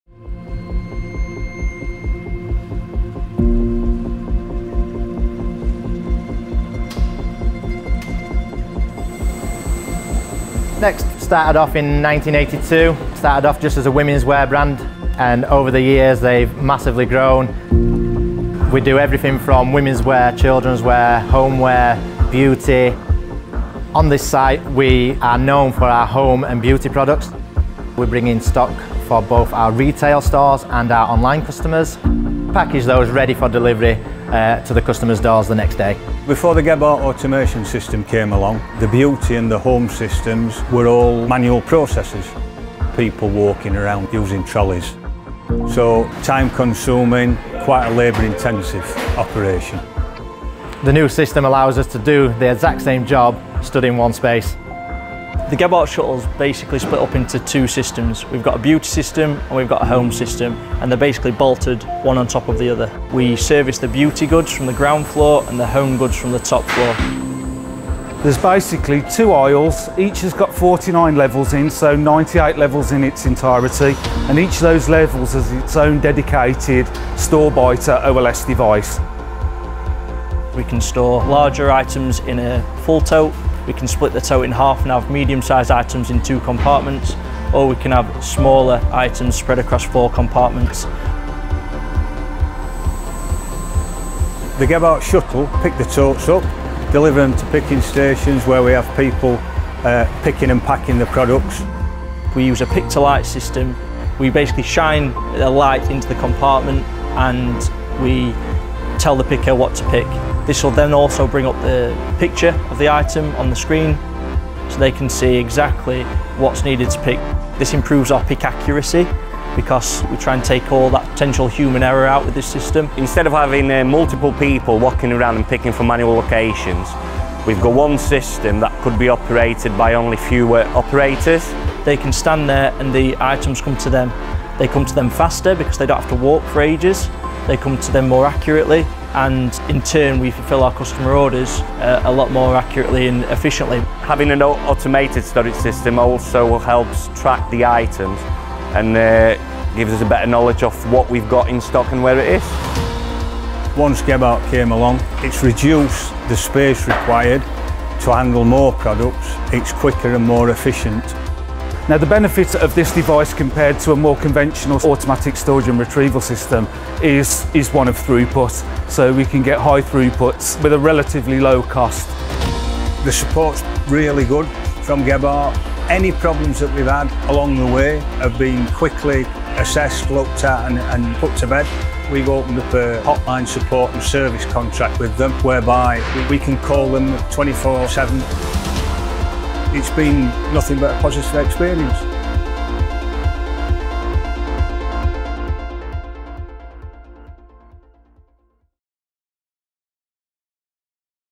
Inside NEXT's Distribution Centre: Managing Thousands of Items Daily
Distributing thousands of orders each day is no mean feat. Watch to learn more about the warehouse automation powering daily operations at NEXT.

System created, manufactured, and installed by GEBHARDT Intralogistics UK.

Product rundown:
GEBHARDT StoreBiter® OLS – One-level shuttle system for automatic storage and retrieval.
GEBHARDT StoreWare – Warehouse software for optimising automated processes.
System 300 Conveyor System – Conveyors for totes, cartons, and trays.